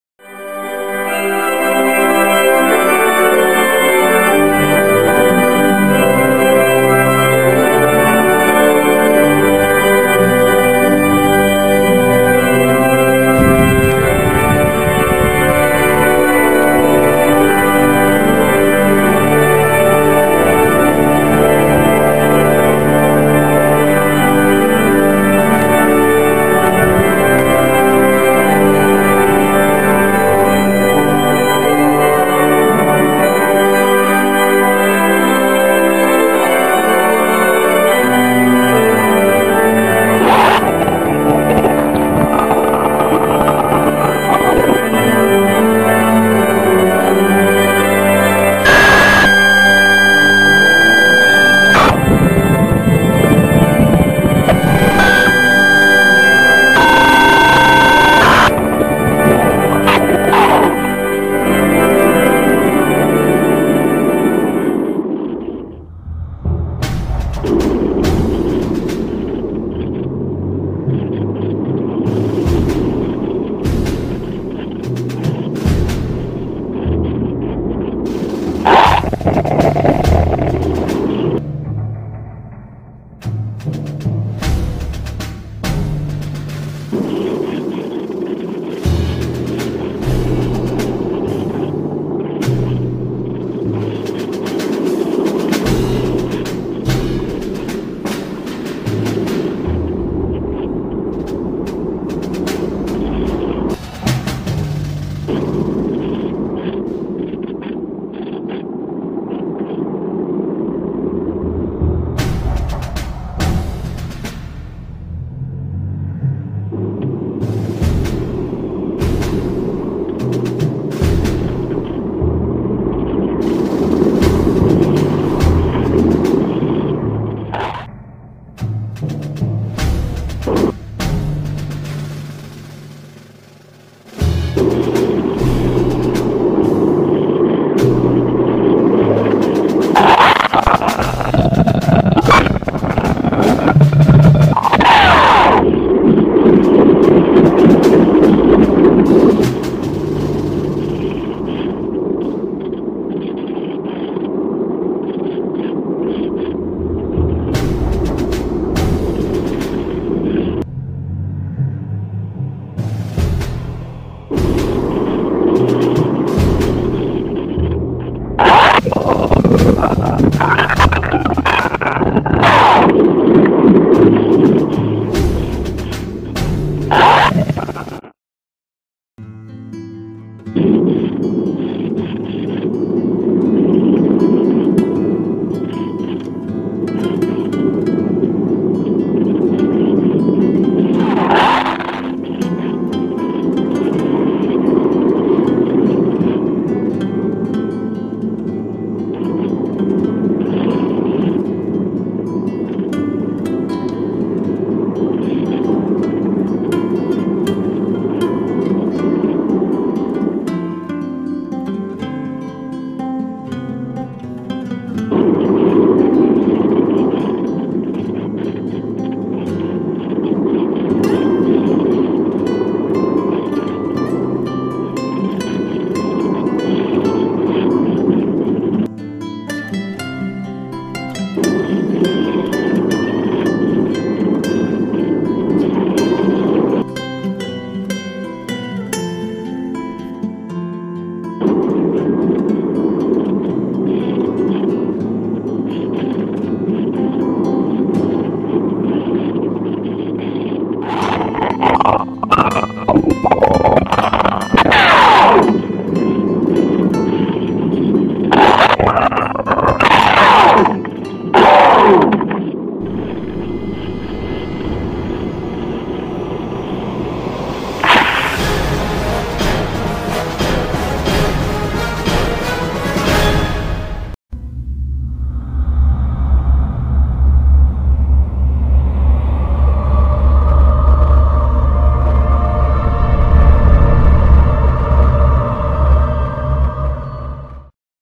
Śrem , Zbrudzewo z lotu ptaka - loty RC modelarzy
Nic nadzwyczajnego, ot, latanko.
3 modele: Blizzard, Su-29 i Moto-Koliber.